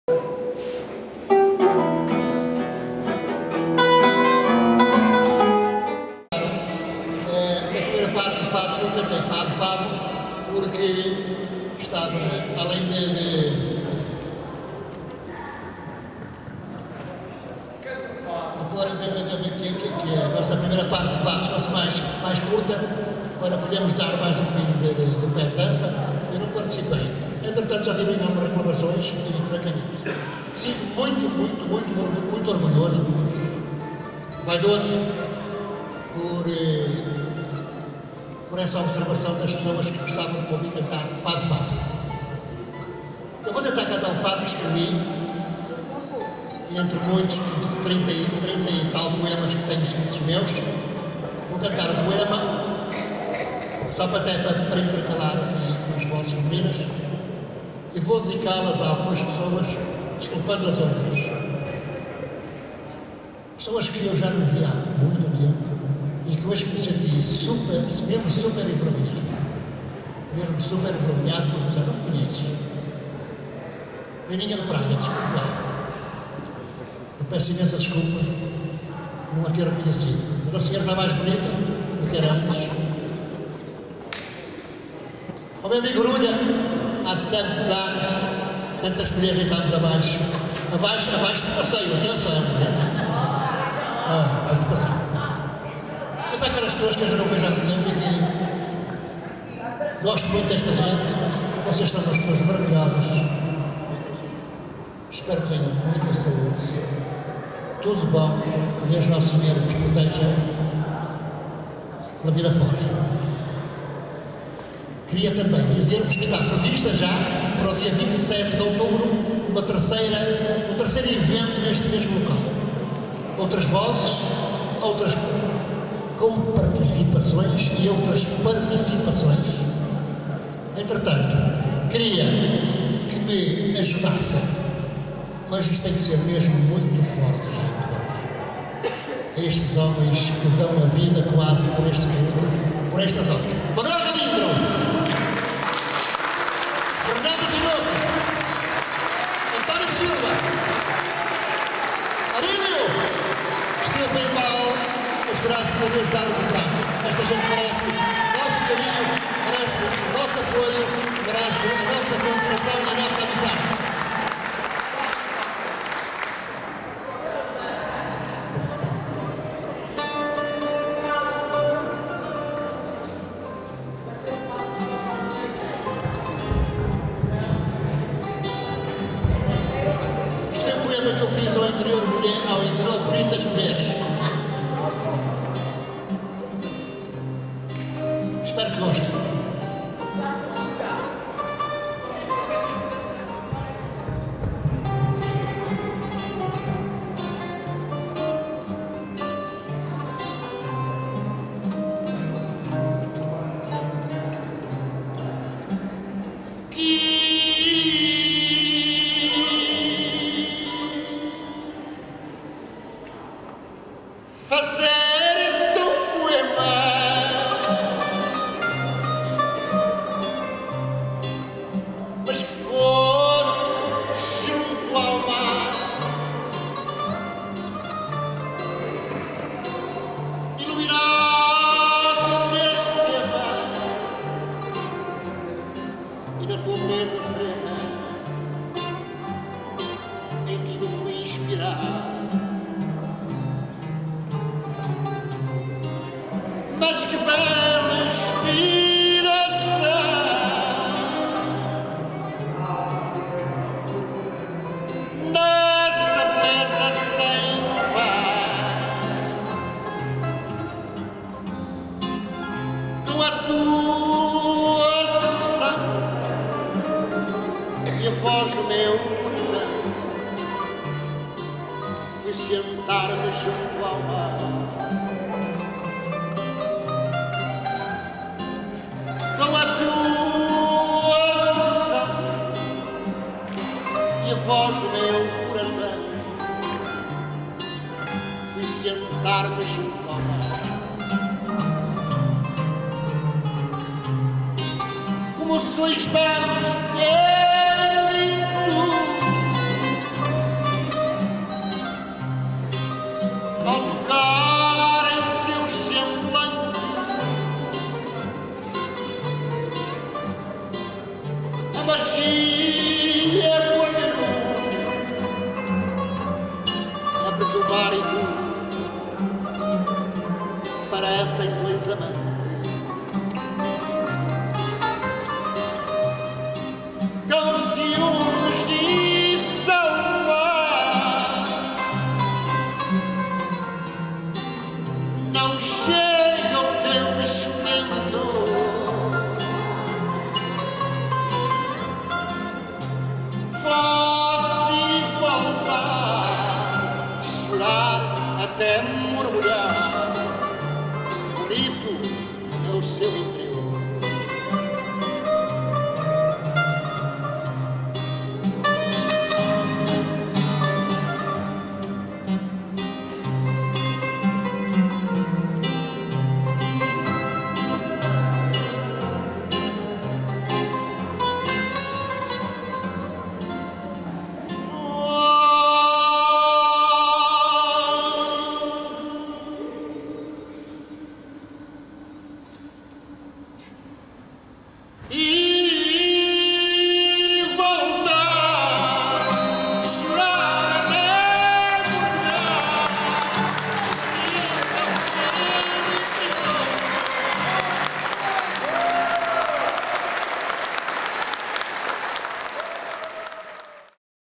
Vitor Rodrigues Fadista
Bonita noite de fados no Pavilhão do Leça da palmeira  Guitarra Portuguesa Nelito Marques
Viola de fado Joana Almeida